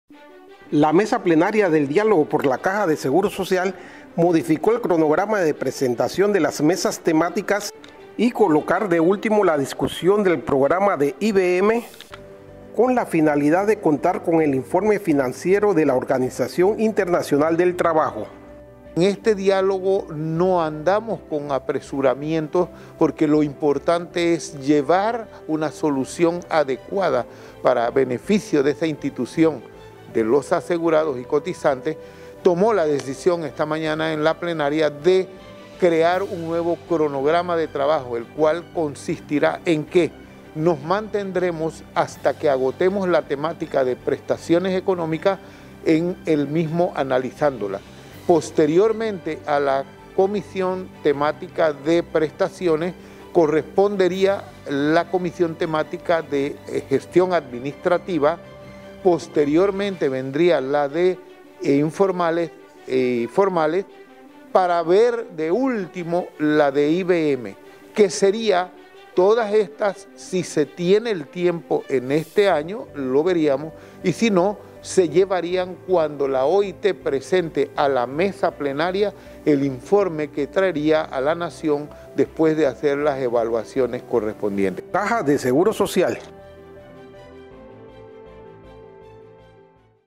Diálogo por la CSS Modifica Cronograma de Trabajo
La Mesa Plenaria del Diálogo por la Caja de Seguro Social aprobó este lunes, 25 de octubre, modificar el cronograma de presentaciones de los informes de las mesas temáticas, con la finalidad de discutir las propuestas de la Mesa de Invalidez, Vejez y Muerte cuando la Organización Internacional de Trabajo presente su informe sobre el estado financiero de la CSS.

El facilitador general del Diálogo, Prof. Joaquín Villar García, informó que las presentaciones de las Mesas Temáticas, que se habían establecido en primer lugar la de Prestaciones Económica, Enfermedad y Maternidad y Riesgos Profesionales; en segundo lugar la de Invalidez, Vejez y Muerte; de tercero la de Administración y por último, el de Formales e Informales, se modificó su orden de la siguiente forma:

Prestaciones Económicas, Enfermedad y Maternidad y Riesgos Profesionales
Administración
Formales e Informales
Invalidez, Vejez y Muerte